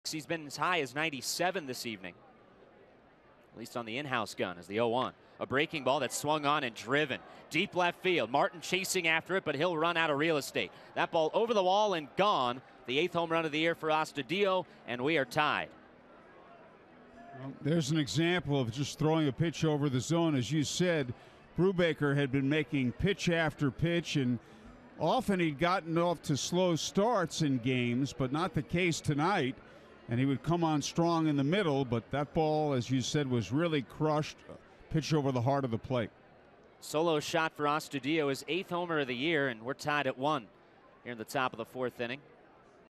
Astudillo's solo home run for the Red Wings
7/25/18: Willians Astudillo blasts off on a solo home run for Rochester